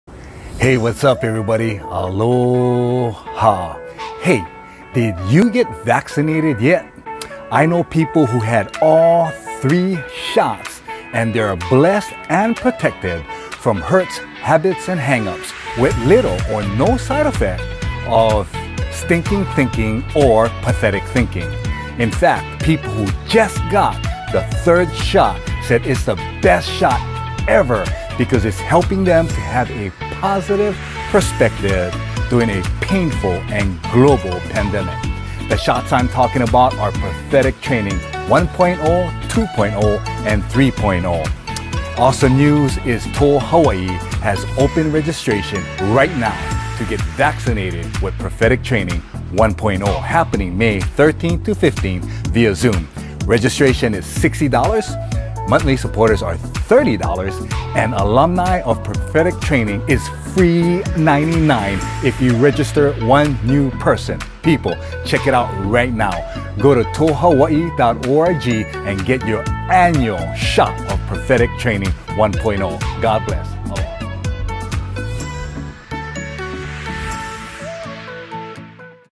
Join us for Prophetic Training 1.0 with Dano McCollam & Bethany Hicks
How to Hear God’s voice for yourself and the world around you.
This Prophetic Training provides the first building blocks for establishing you in the gift of prophecy. You will discover multiple ways to receive prophetic information and also various ways to deliver prophetic words. At the end of this training, we believe you will be more confident in your ability to hear and respond to the voice of God.

Content covered:
1. Prophetic Values and Protocols
2. Ways To Receive
3. Ways to Deliver prophetic information

Dates:  May 13 - 15, 2021
Times:
May 13, 6 - 8:15 pm,
May 14, 6 - 8:30 pm
May 15, 9 am - 12:30 pm
(Backstage Event for Supporters 12:40-1:10)

Online Platform: Online via Zoom
*We will NOT meet in person.

Registration: $60
Monthly Supporters: $30
Alumni: FREE registration if they invite at least one new registered attendee (someone who’s never taken any of the Prophetic Training courses before)

Registration closes on May 12 at Midnight